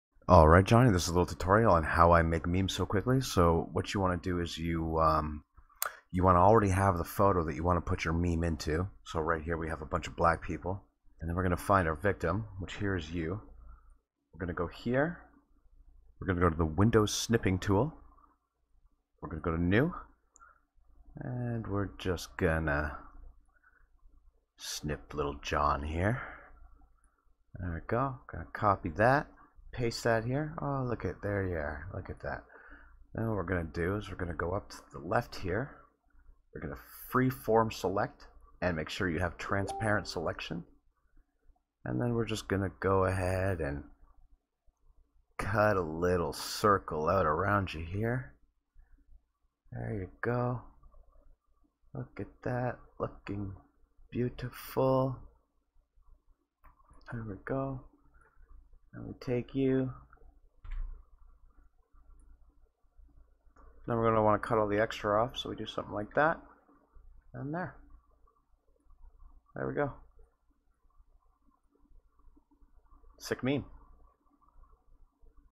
john.mp4
John - an ms paint tutorial.

an extremely in-depth look at microsoft PAINT. THE BEST PHOTO EDITTING SOFTWARE FOR FREE